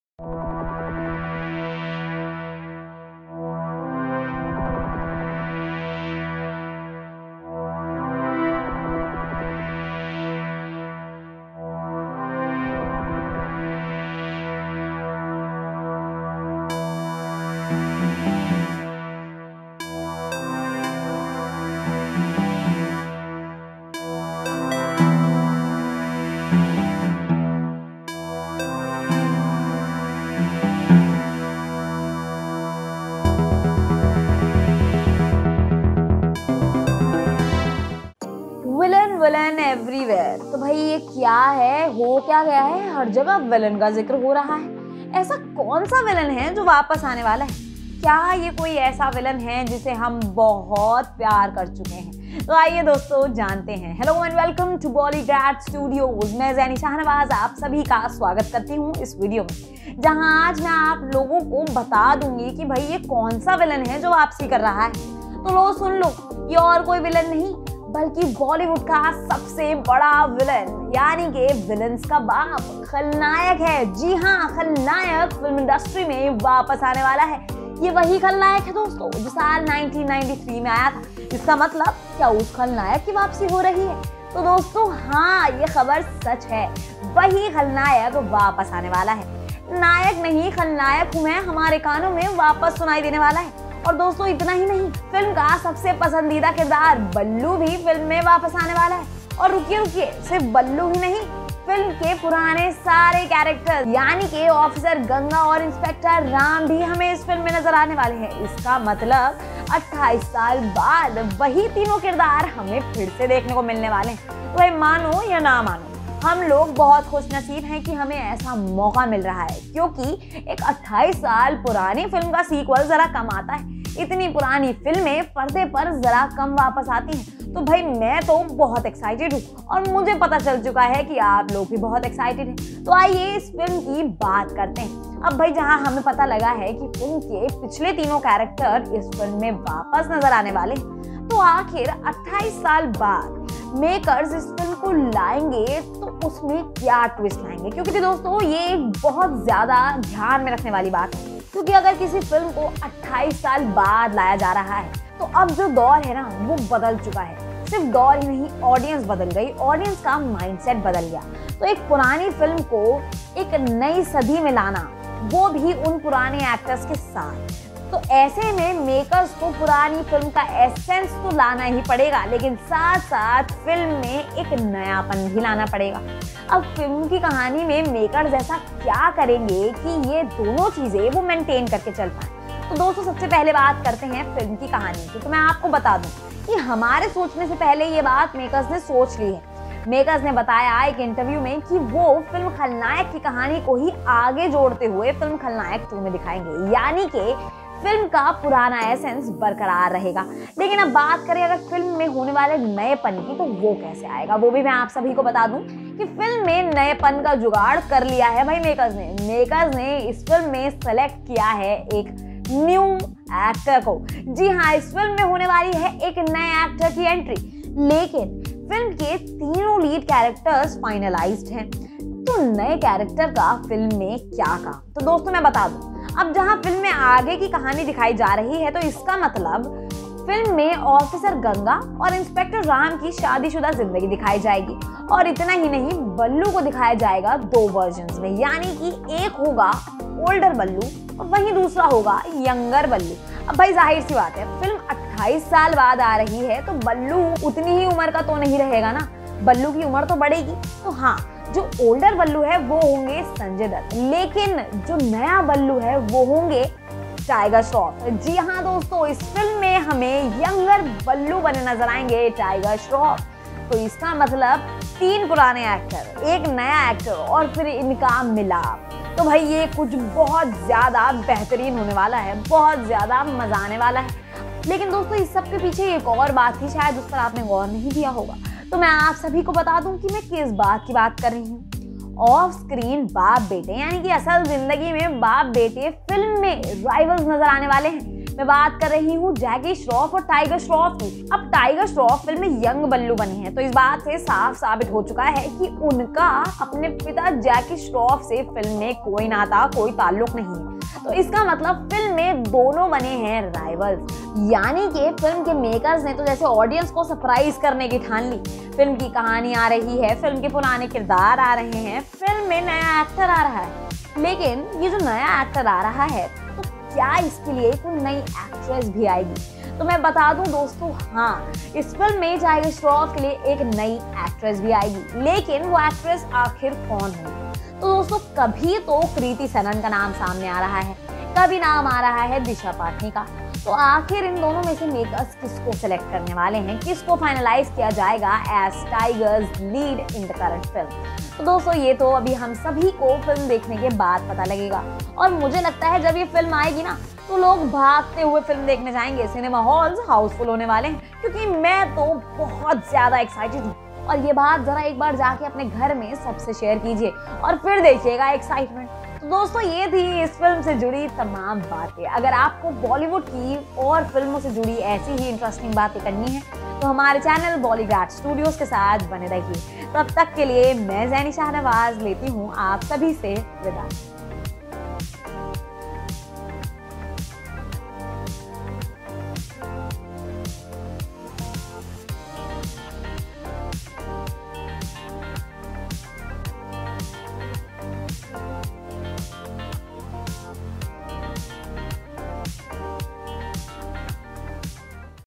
Khalnayak 2 | 33 Interesting Facts I Sanjay Dutt I Madhuri I Jackie Shroff IAnupam |Tiger Shroff
Some of the most Interesting and Amazing full movie facts of Khalnayak 2 have been told in this video.
What will be the story of khalnayak 2 ?
Which is our favourite villain coming back for movie " Khalnayak 2 " ??

Action star Tiger Shroff has made a strong mark in Bollywood with his amazing acting skills. The actor is not only loved for his acting but also for his great dance moves and jaw-dropping action sequences.

The Heropanti star won the hearts of millions with movies like the Baaghi series, War, and Student of the Year 2.

Now, the actor will be seen in yet another action thriller: the sequel of one of the most loved movies, Khalnayak.

Khalnayak released in 1993 and was a superhit. Directed by showman Subhash Ghai, it starred Sanjay Dutt, Jackie Shroff, and Madhuri Dixit in the leads. Sanjay received immense love for this movie, and soon, we shall see Tiger in its sequel.

Anchor and Script - Zaini Shahnawaz
Editor - Shubham Madavi